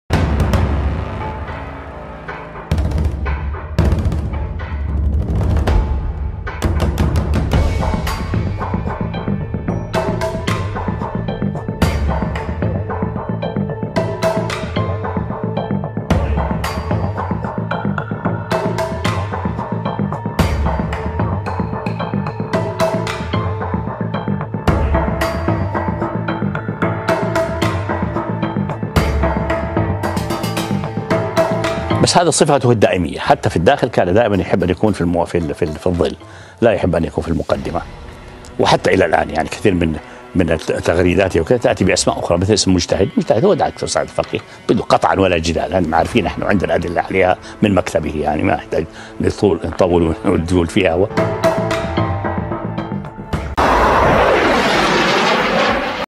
محمد المسعري يفضح سعد الفقيه ويكشف حقيقة حساب مجتهد
حيث صرح بذلك المسعري في لقاء له بأن حساب مجتهد صاحبه سعد الفقيه